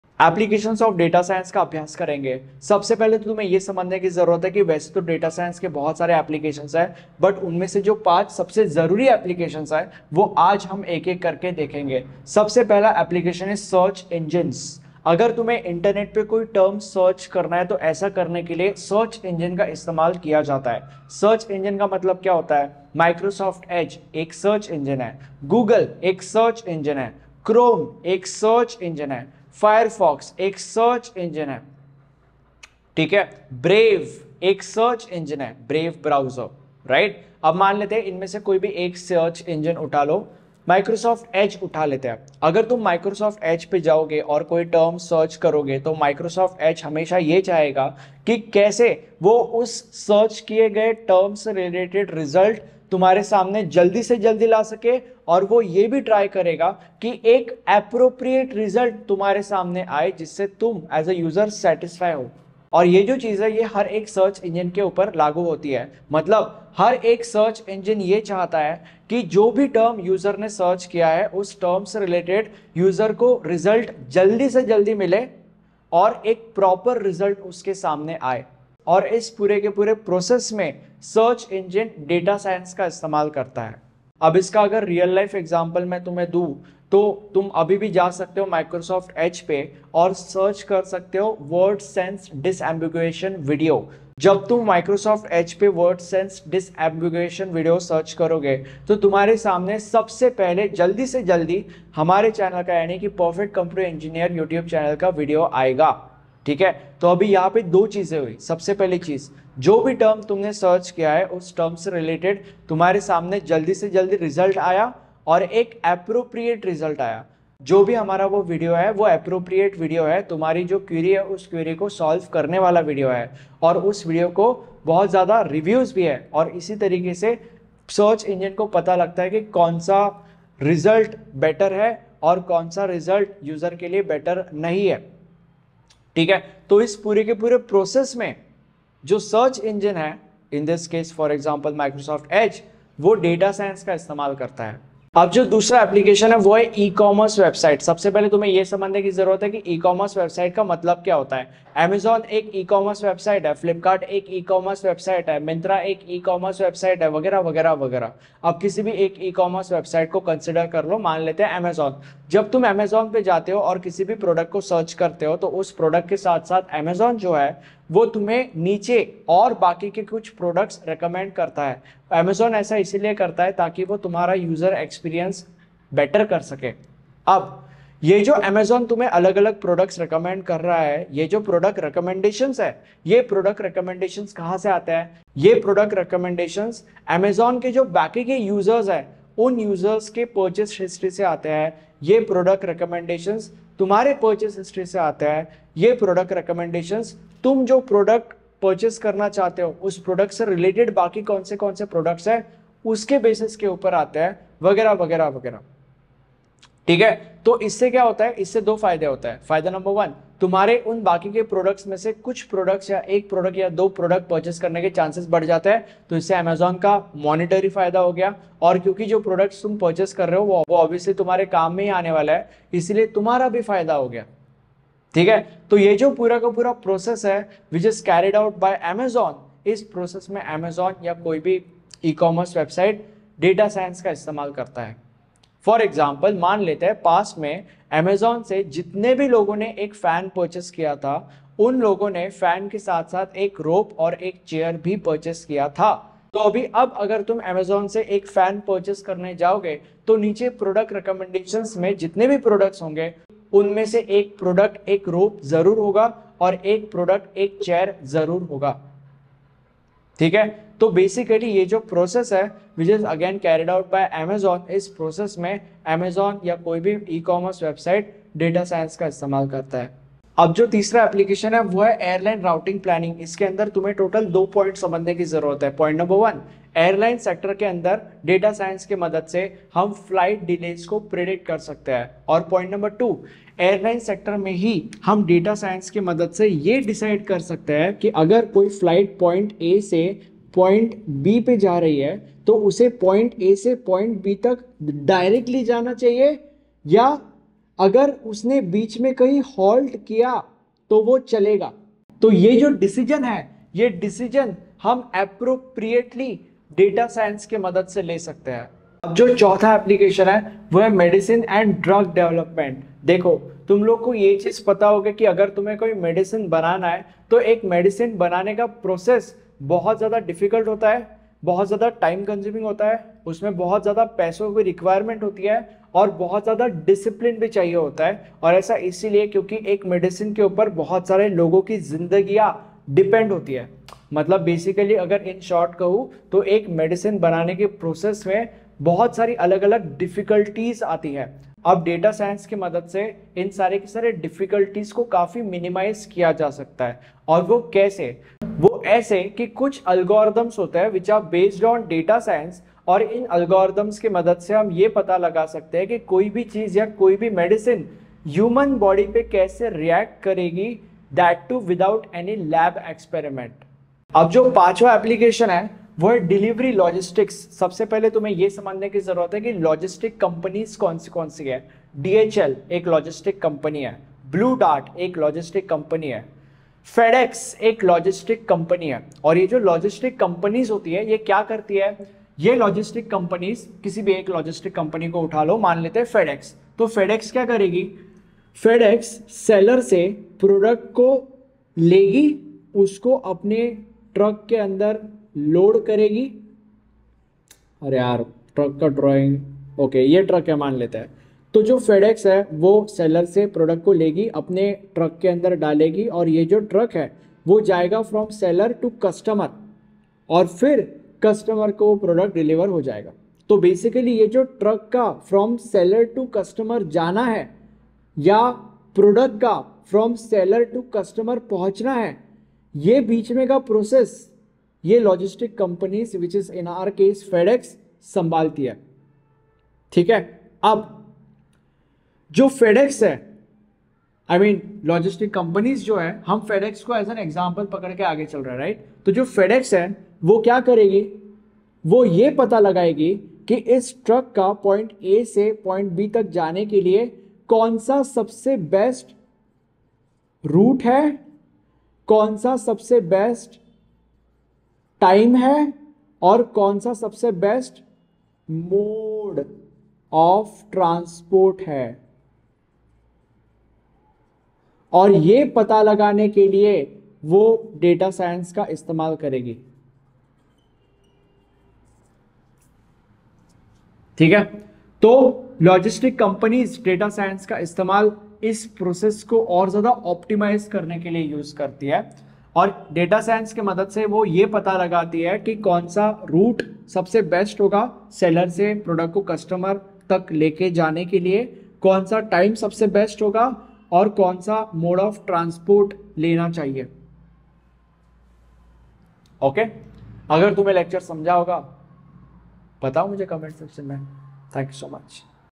Applications of Data Science
Applications of Data Science in Applied Data Science in Hindi is the topic taught in this lecture. This lecture talks about different applications like:
0:10 Search Engines
2:20 E-commerce
4:30 Airline routing planning
5:05 Medicine & drug development
6:00 Delivery logistics

Purchase updated notes right now,